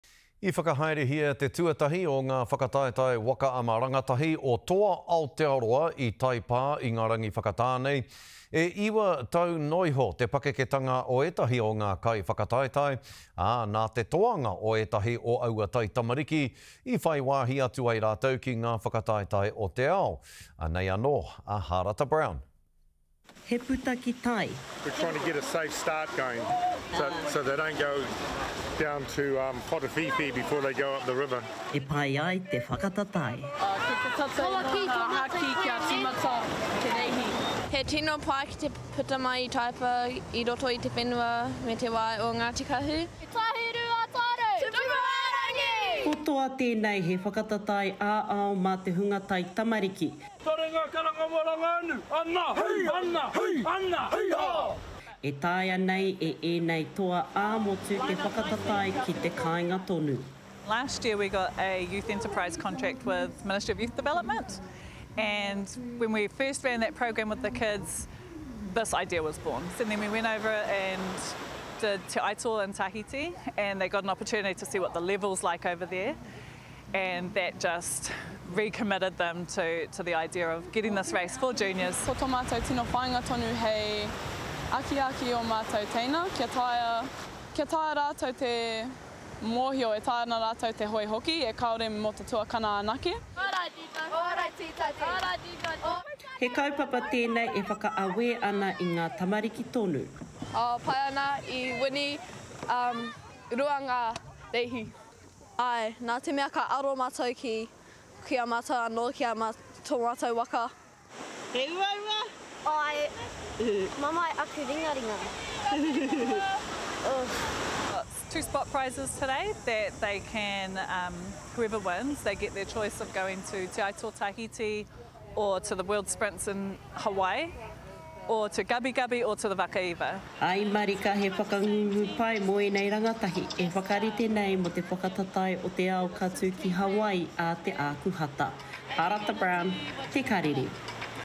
First ever Toa NZ Junior Waka Ama Championship takes place
The very first Toa NZ junior waka ama championship based out of Taipa, Doubtless Bay took place over the weekend. Children as young as 9 took to the seas with some nabbing international prizes to compete overseas. Harata Brown reports.